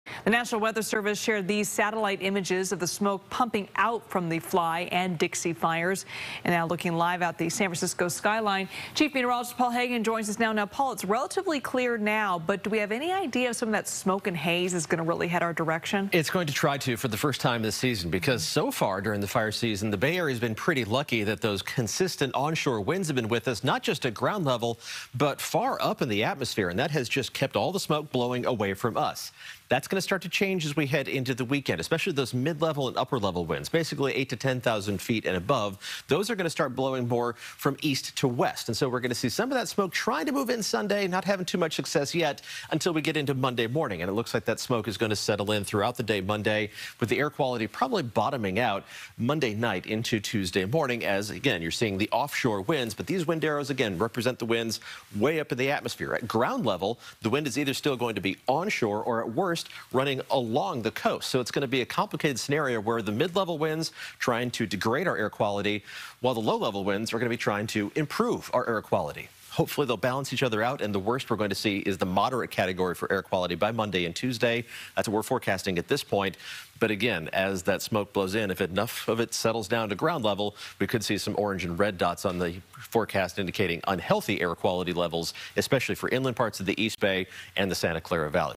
NorCal Wildfire Smoke Could Impact Bay Area Air Quality Starting Sunday Night
Paul Heggen explains how the Bay Area may have to deal with air quality impacts from wildfire smoke for the first time this season starting Sunday (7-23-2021)